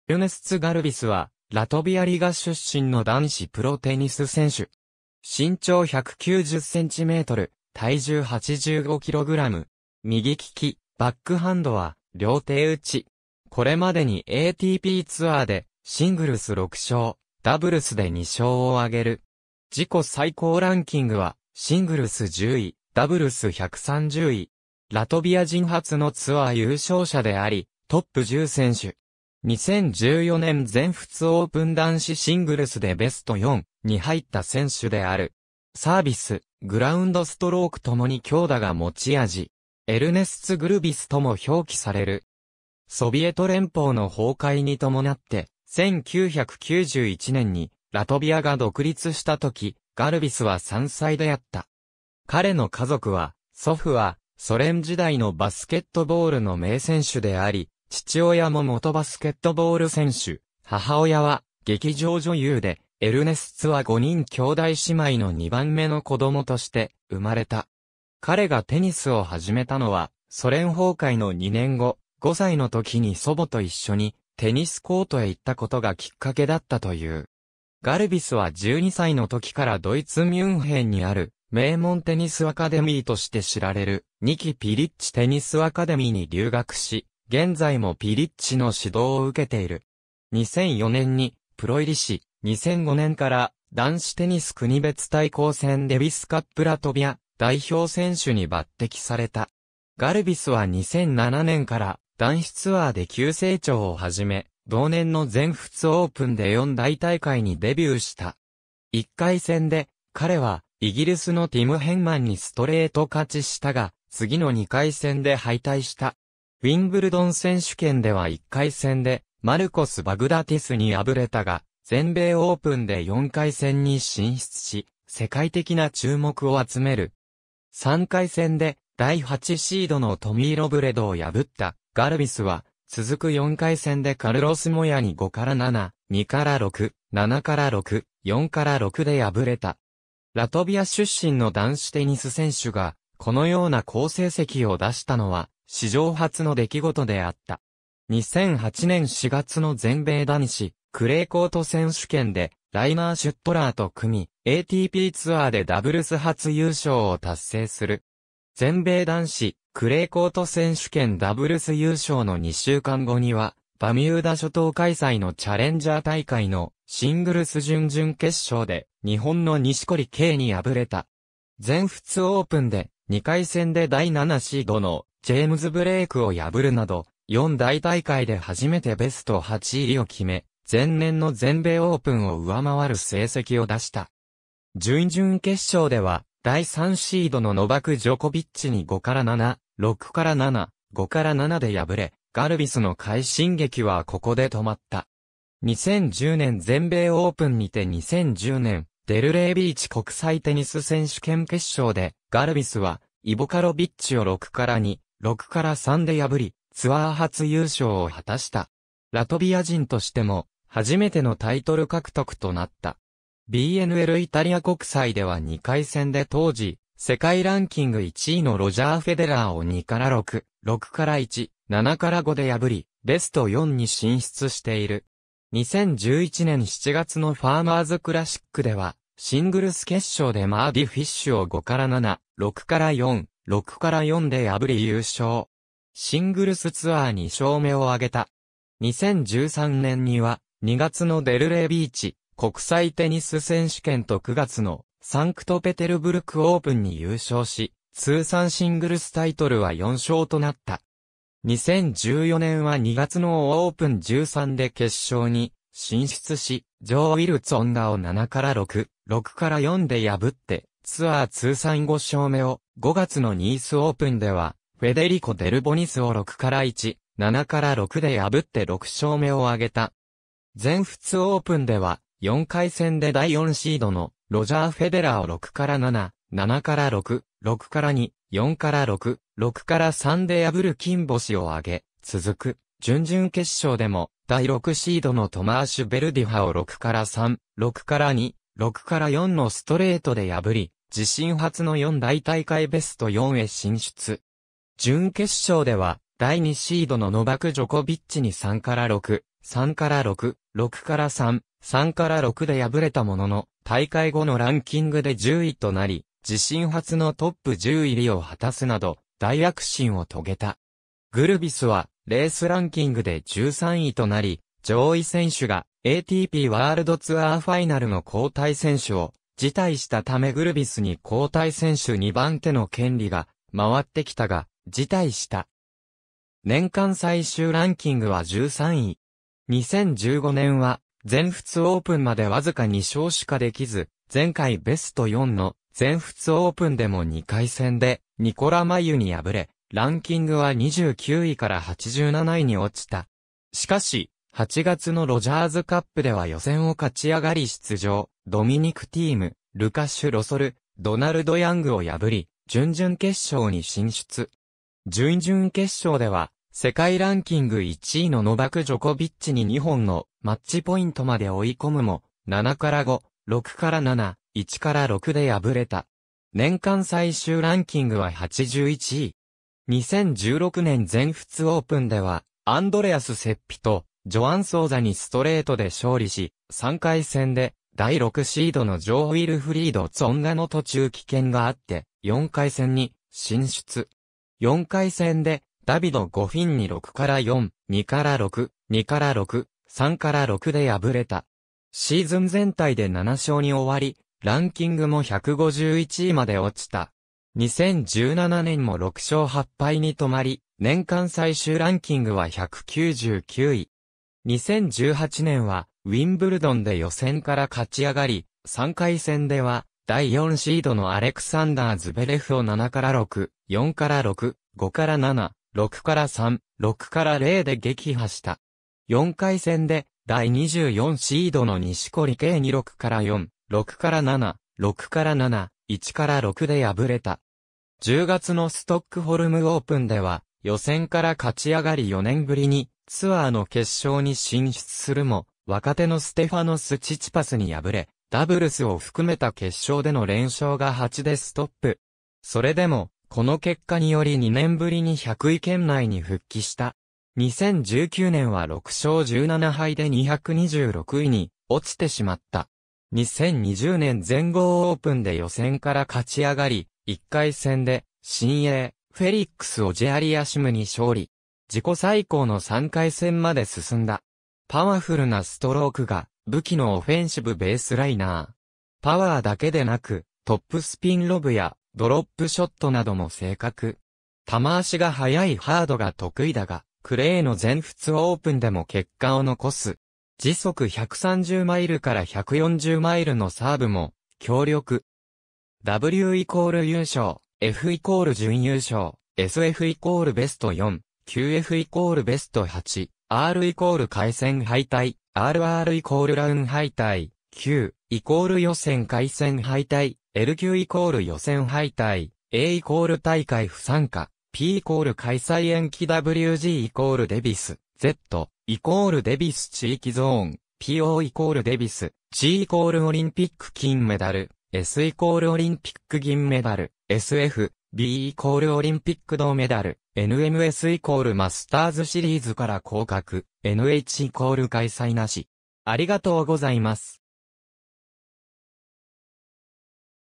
エルネスツ・ガルビス
エルネスツ・ガルビス（Ernests Gulbis, IPA: [ˈærnest͡s ˈɡulbis]; 1988年8月30日 - ）は、ラトビア・リガ出身の男子プロテニス選手。
右利き、バックハンドは両手打ち。
これまでにATPツアーでシングルス6勝、ダブルスで2勝を挙げる。
ラトビア人初のツアー優勝者であり、トップ10選手。
2014年全仏オープン男子シングルスでベスト4に入った選手である。
サービス、グラウンドストロークともに強打が持ち味。
「エルネスツ・グルビス」とも表記される。
ソビエト連邦の崩壊にともなって、1991年にラトビアが独立した時、ガルビスは3歳であった。
彼の家族は、祖父はソ連時代のバスケットボールの名選手であり、父親も元バスケットボール選手、母親は劇場女優で、エルネスツは5人兄弟姉妹の2番目の子供として生まれた。
彼がテニスを始めたのは、ソ連崩壊の2年後、5歳の時に祖母と一緒にテニスコートへ行ったことがきっかけだったという。
ガルビスは12歳の時からドイツ・ミュンヘンにある名門テニスアカデミーとして知られる「ニキ・ピリッチ・テニスアカデミー」に留学し、現在もピリッチの指導を受けている。
2004年にプロ入りし、2005年から男子テニス国別対抗戦デビスカップラトビア代表選手に抜擢された。
ガルビスは2007年から男子ツアーで急成長を始め、同年の全仏オープンで4大大会にデビューした。
1回戦で、彼はイギリスのティム・ヘンマンにストレート勝ちしたが、次の2回戦で敗退した。
ウィンブルドン選手権では1回戦でマルコス・バグダティスに敗れたが、全米オープンで4回戦に進出し、世界的な注目を集める。
3回戦で第8シードのトミー・ロブレドを破ったガルビスは、続く4回戦でカルロス・モヤに5-7, 2-6, 7-6, 4-6で敗れた。
ラトビア出身の男子テニス選手がこのような好成績を出したのは史上初の出来事であった。
2008年4月の全米男子クレーコート選手権でライナー・シュットラーと組み、ATPツアーでダブルス初優勝を達成する。
全米男子クレーコート選手権ダブルス優勝の2週間後には、バミューダ諸島開催のチャレンジャー大会のシングルス準々決勝で日本の錦織圭に敗れた。
全仏オープンで、2回戦で第7シードのジェームズ・ブレークを破るなど、4大大会で初めてベスト8入りを決め、前年の全米オープンを上回る成績を出した。
準々決勝では第3シードのノバク・ジョコビッチに5-7, 6-7, 5-7で敗れ、ガルビスの快進撃はここで止まった。
2010年全米オープンにて 2010年デルレイビーチ国際テニス選手権決勝でガルビスはイボ・カロビッチを6-2, 6-3で破りツアー初優勝を果たした。
ラトビア人としても初めてのタイトル獲得となった。
BNLイタリア国際では2回戦で当時世界ランキング1位のロジャー・フェデラーを2-6, 6-1, 7-5で破りベスト4に進出している。
2011年7月のファーマーズ・クラシックではシングルス決勝でマーディ・フィッシュを5-7, 6-4, 6-4で破り優勝。
シングルスツアー2勝目を挙げた。
2013年には、2月のデルレイビーチ国際テニス選手権と9月のサンクトペテルブルク・オープンに優勝し、通算シングルスタイトルは4勝となった。
2014年は2月のオープン13で決勝に進出しジョー＝ウィルフリード・ツォンガを7-6(5), 6-4で破ってツアー通算5勝目を、5月のニース・オープンではフェデリコ・デルボニスを6-1, 7-6(5)で破って6勝目を挙げた。
全仏オープンでは、4回戦で第4シードのロジャー・フェデラーを6-7(5), 7-6(3), 6-2, 4-6, 6-3で破る金星を挙げ、続く準々決勝でも第6シードのトマーシュ・ベルディハを6-3, 6-2, 6-4のストレートで破り、自身初の4大大会ベスト4へ進出。
準決勝では第2シードのノバク・ジョコビッチに3-6, 3-6, 6-3, 3-6で敗れたものの、大会後のランキングで10位となり、自身初のトップ10入りを果たすなど大躍進を遂げた。
グルビスはレースランキングで13位となり、上位選手がATPワールドツアーファイナルの交替選手を辞退したためグルビ...